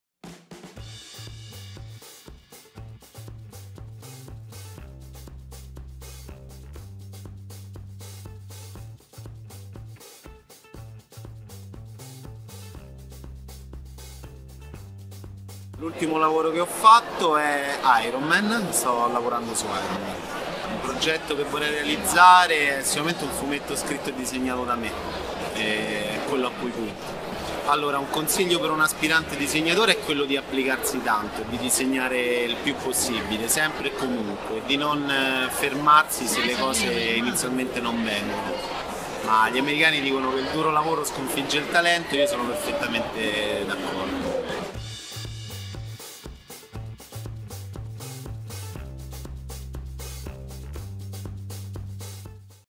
Tre domande a... STEFANO CASELLI
Tre domande a... Stefano Caselli, disegnatore Marvel e insegnante della Scuola Romana dei Fumetti.
Intervista realizzata a Romics ottobre 2017